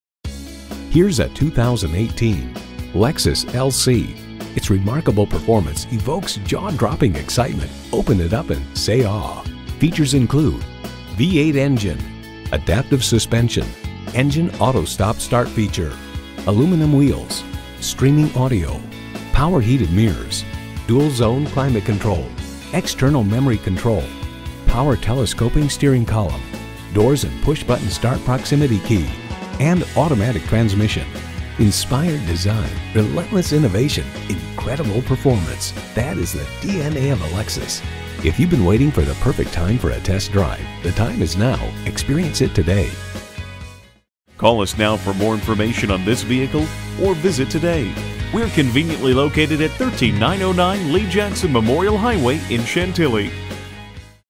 Used 2018 Lexus LC Chantilly Dale City VA DC, MD LP190564 - SOLD
Year: 2018
Make: Lexus
Model: LC
Trim: LC 500 RWD
Engine: 303 L 8 Cyl. Cyl.
Color: Nightfall Mica
Interior: Tan
Mileage: 3009
Stock #: LP190564
VIN: JTHHP5AY7JA001379

Address:
13909 Lee Jackson Memorial Hwy